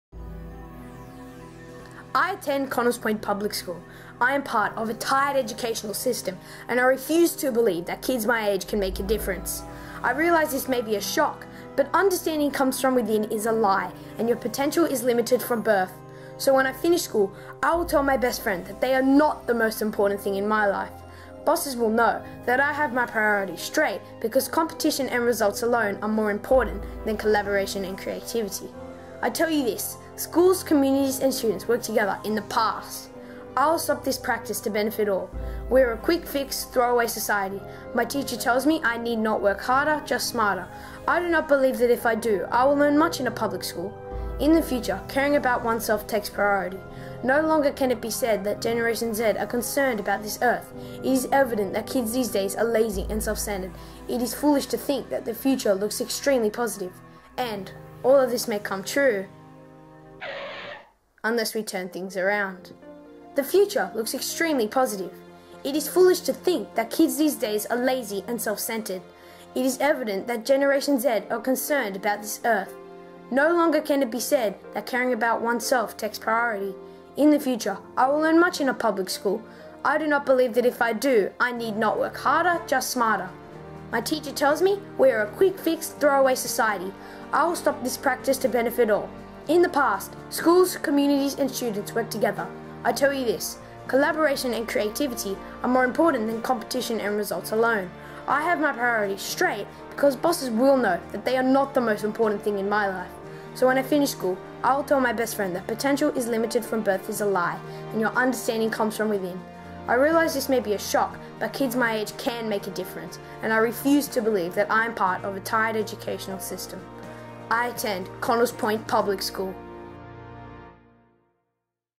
Connells Point PS Reverse Poem
This reverse poem was first performed at Connell Point Public Schools 2015 Performing Arts Showcase. The poem was written by Paul Cohen (a teacher) and recited by Noah in Year 6. The poem took inspiration from the poem ‘The Lost Generation’ by Jonathan Reed. If you would like to learn more about Connells Point Public School in Sydney (Australia), please visit the school . We hope that you enjoy the poem!

I attend Connells Point Public School.
I am part of a tired educational system.
And I refuse to believe that
kids my age can make a difference.
I realise this may be a shock, but,
“understanding comes from within”
Is a lie, and your
potential is limited from birth.
So when I finish school, I will tell my best friend that
they are not the most important thing in my life.
Bosses will know that
I have my priorities straight because
Competition and results alone
are more important than
Collaboration and creativity
I tell you this:
Schools, communities and students worked together
In the past,
I will stop this practice to benefit all.
we are a quick fix, throw-away society;
My teacher tells me
I need not work harder, just smarter.
I do not believe that if I do
I will learn much in a public school.
In the future,
Caring about one’s self takes priority.
No longer can it be said that
Generation Z are concerned about this Earth.
It is evident that
kids these days are lazy and self-centred.
It is foolish to think that
The future looks extremely positive.

And, all of this may come true unless we turn things around…